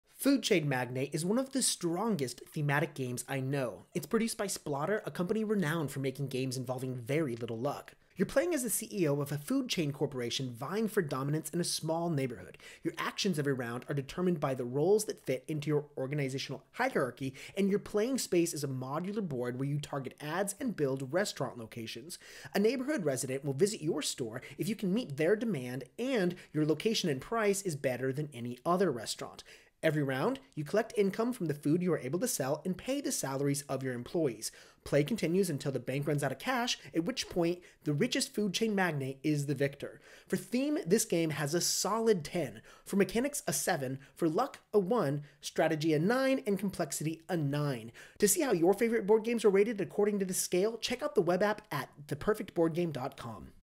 Food Chain Magnate Review in 60 Seconds
Food Chain Magnate is a great game, but for the price tag, is it worth it? In this video I describe what you can expect, how the game is rated according to the Perfect Boardgame scale and who might be interested in such a heavy, strategic game.

Perfect Boardgame Food Chain Magnate  Ratings:
Theme - 10
Mechanics - 7
Luck - 1
Strategy - 9
Complexity - 9